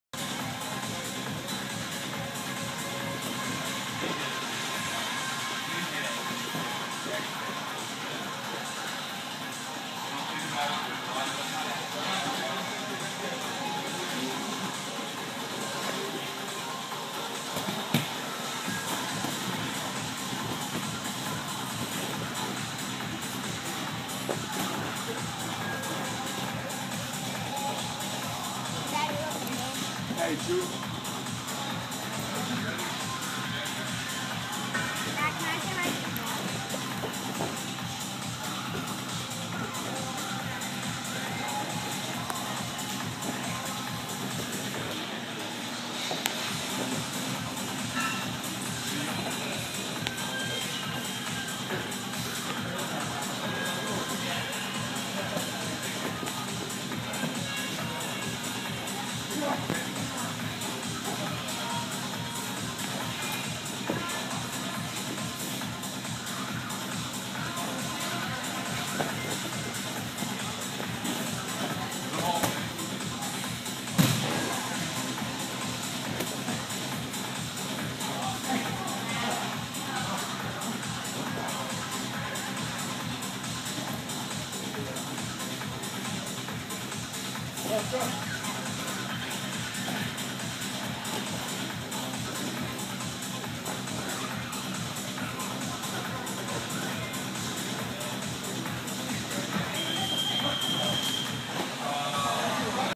Brian Bowles Getting Ready For UFC 160 | The HardCore Gym | Athens MMA
Get a behind the scenes look as Brian Bowles prepares for his UFC 160 fight against George Roop. This is Brian's first fight back since losing to Urijah Faber and he is a man on a mission. Brian is back in The HardCore Gym training hard in hopes of one day getting a shot to win back the UFC bantamweight title which he took from Miguel Torres in devastating fashion.

Tune in to Facebook and watch Brian make his rise back to the top of the 135 pound weight class.

Both got their start at The HardCore Gym in Athens Georgia.